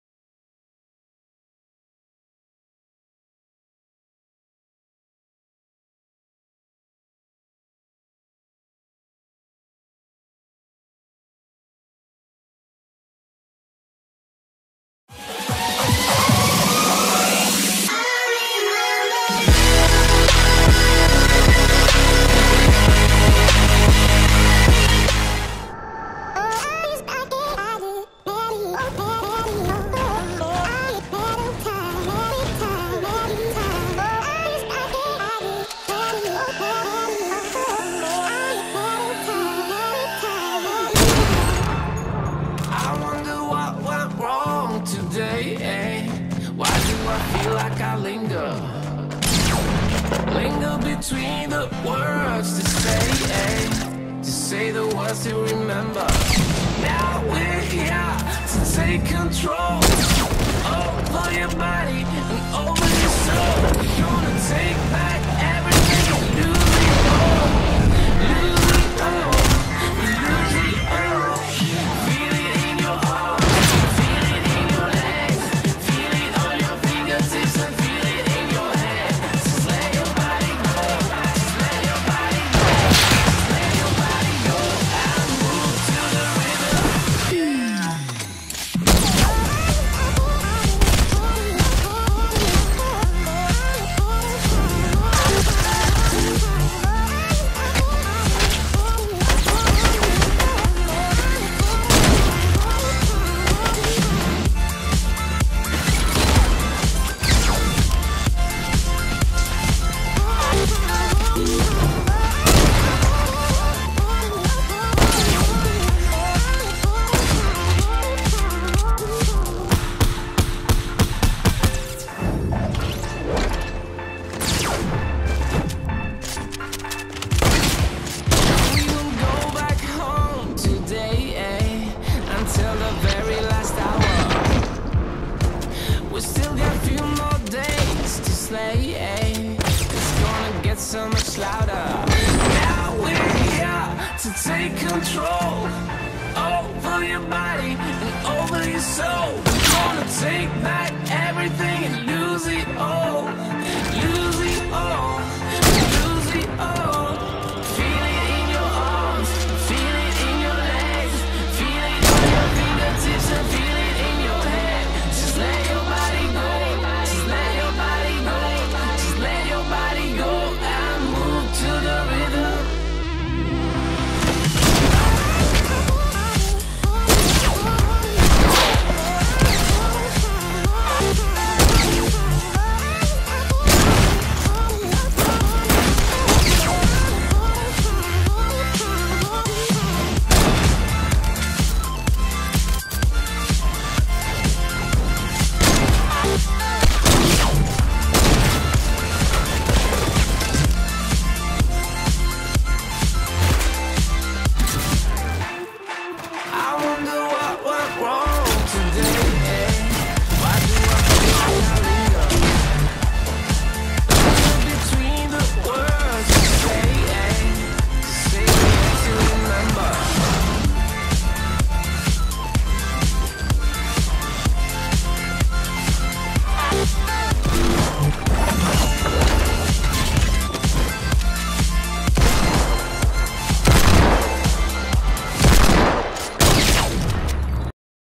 | A PLAYGROUND MONTAGE | XD Frusty | CONSOLE PLAYER |
This took time and effort. The clips from the video shows me (XD Frusty) in many playground games.  I hope you enjoyed.

P.S if the gameplay is bad , it is probably because I was playing on standard.